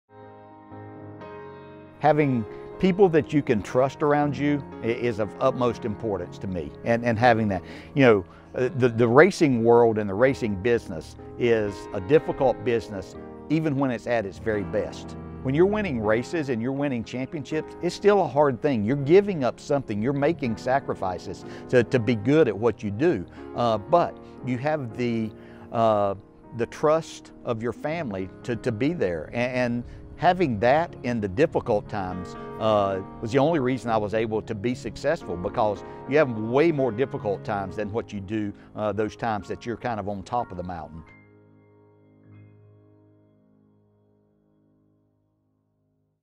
What Trust Means to Dale Jarrett
NASCAR legend Dale Jarrett talks about how trusting his team is what helped him win races and be successful in the racing world. Likewise, people make a difference in Jarrett Logistics’ success, and the foundation of that is trust.